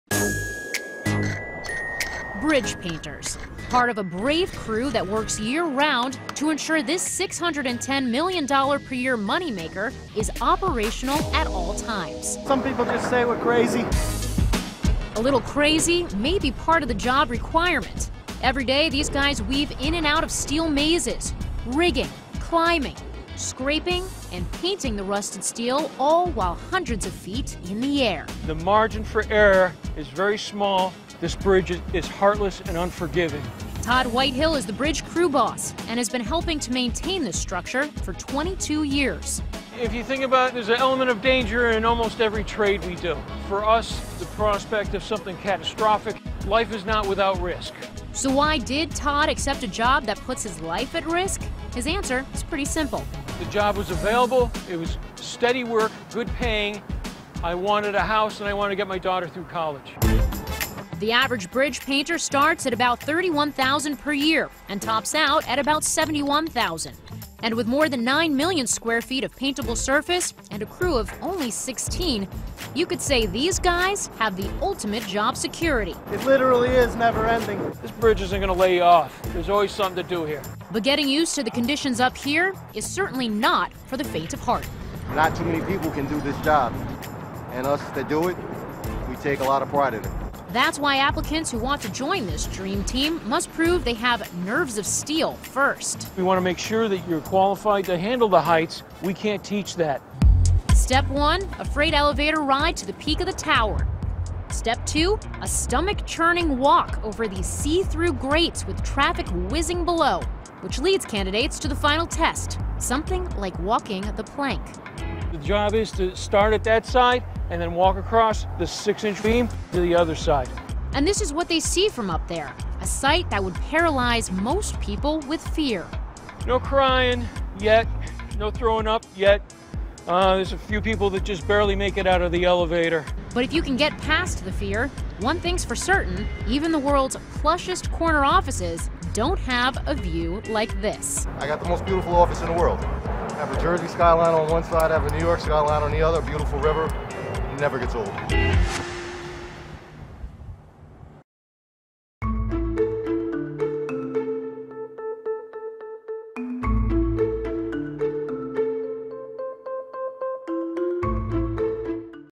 These Painters Have One Of The Scariest Jobs In The World
Todd Whitehill and his team of bridge painters work year-round to ensure the George Washington Bridge is operational at all times.

These Painters Have One Of The Scariest Jobs In The World | CNBC Make It.